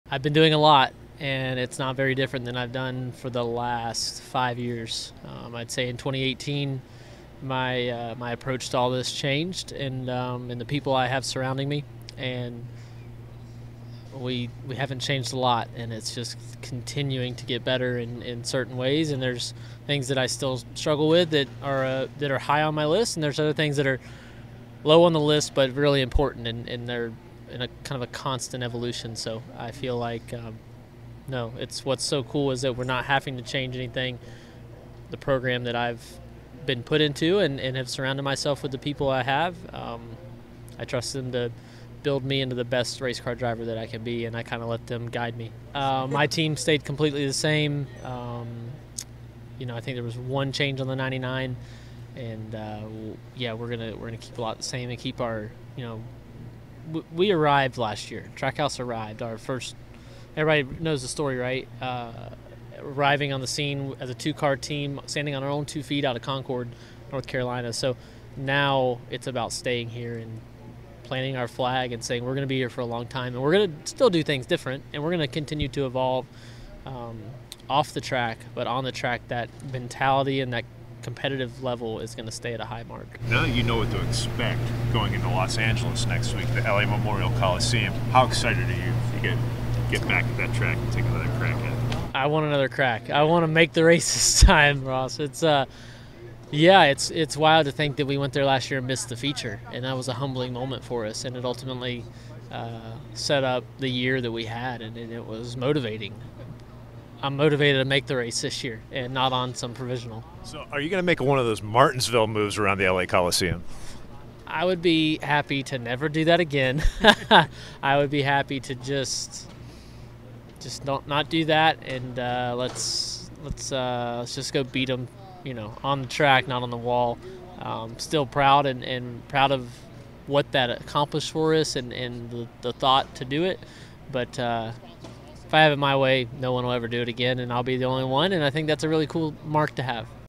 Ross Chastain on Martinsville Hail Melon: "I Would Be Happy To Never Do That Again"
Ross Chastain discusses all things NASCAR.

***thumbnail photo by Chris Graythen/Getty Images courtesy of NASCAR Media